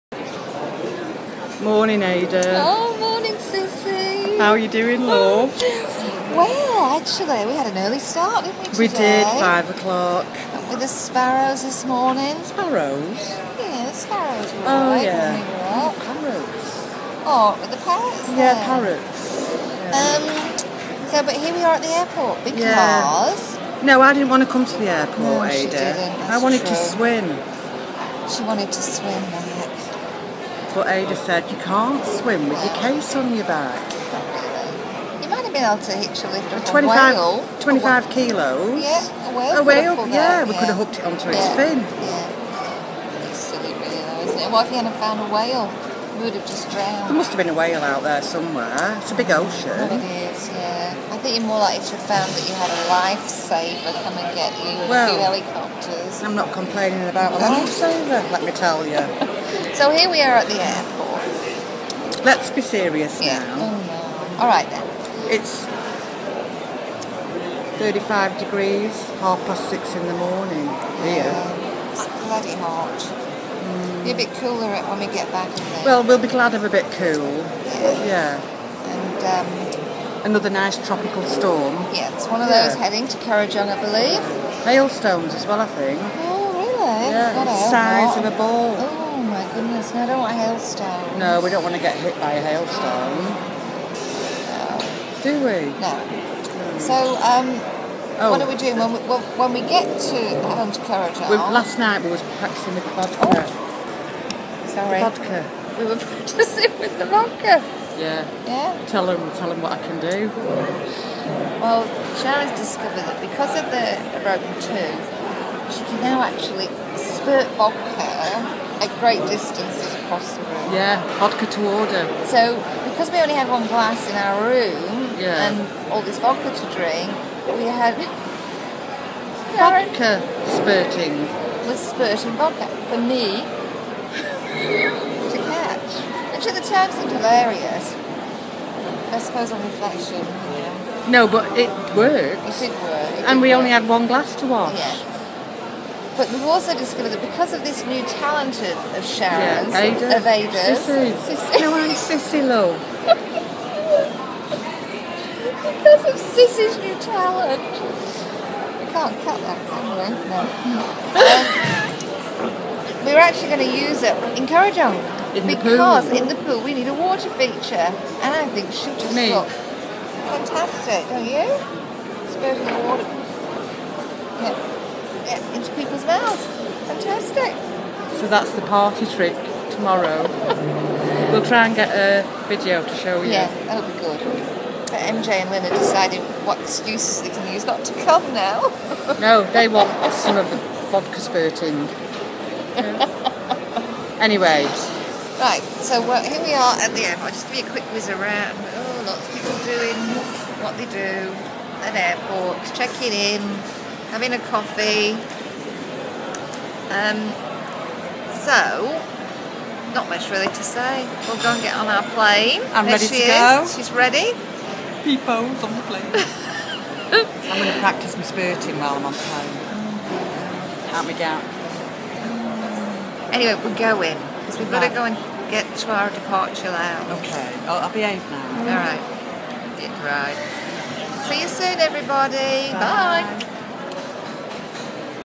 Cissie & Ada do Queensland 11/11/16
Our alter egos had a ball on the Gold Coast Queensland - 2 old chucks having a chat!